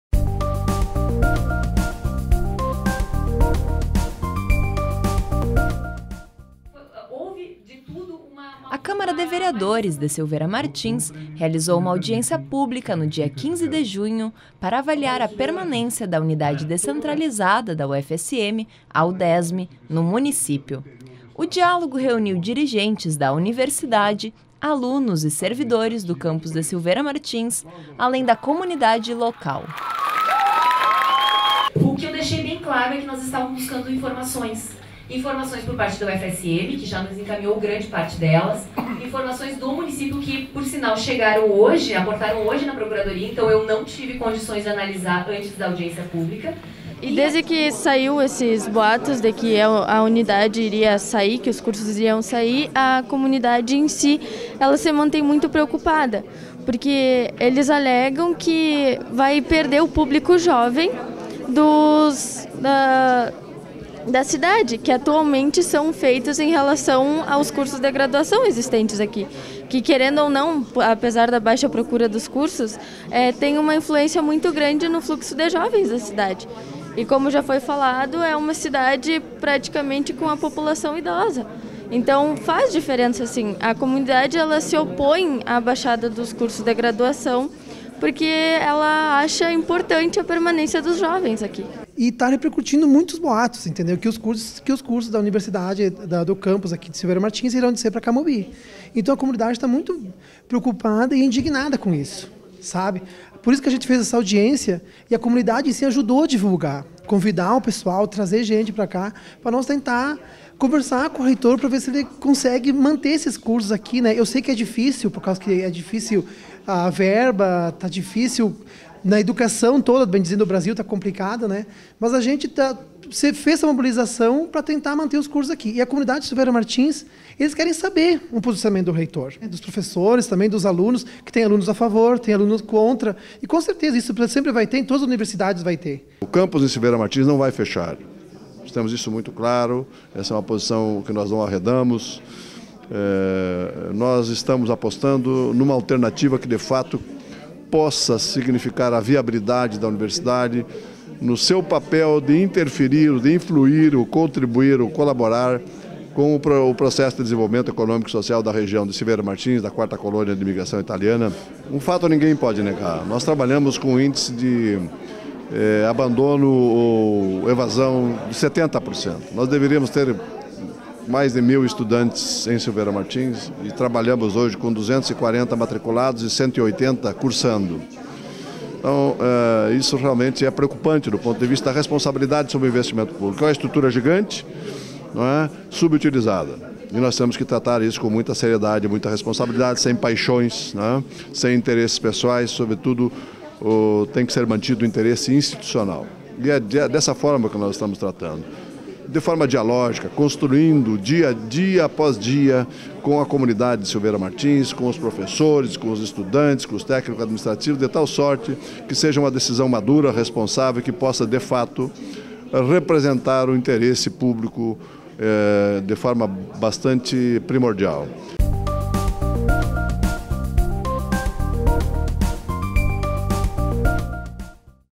Audiência Pública Permanência UDESSM - Acontece na UFSM
O futuro da UFSM em debate público! A comunidade de Silveira Martins está mobilizada para a discussão que definirá a permanência da unidade da UFSM na cidade. Na noite desta quarta-feira, 15 de junho, uma audiência pública reuniu dirigentes da universidade, representantes dos estudantes, dos servidores e de diversos segmentos sociais para debater a questão.